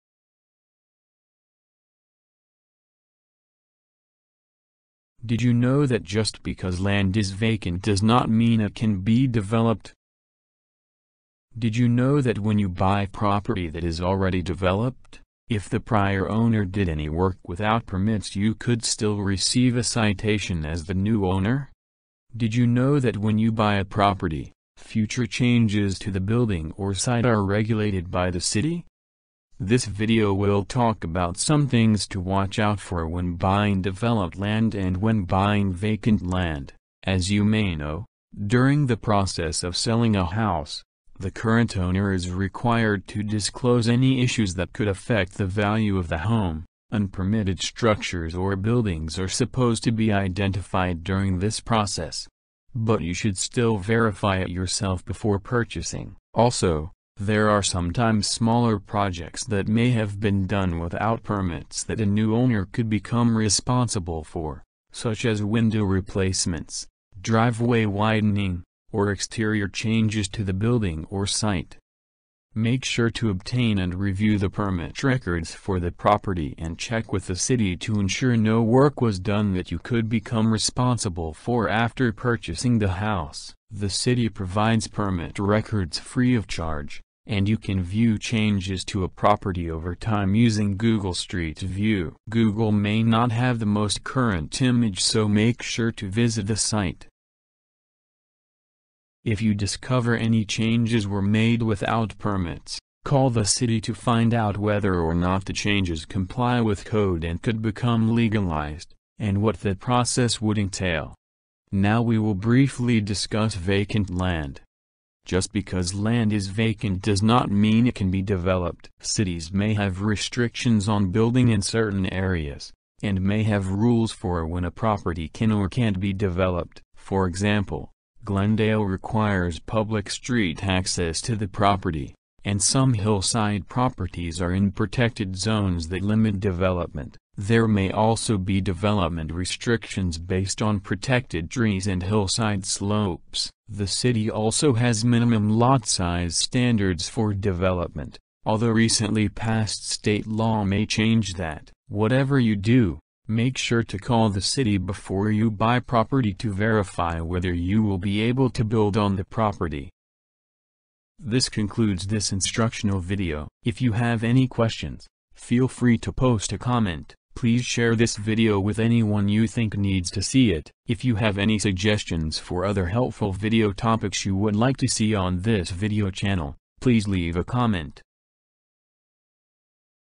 Before Buying Property in Glendale California
Things to watch out for before buying property in Glendale California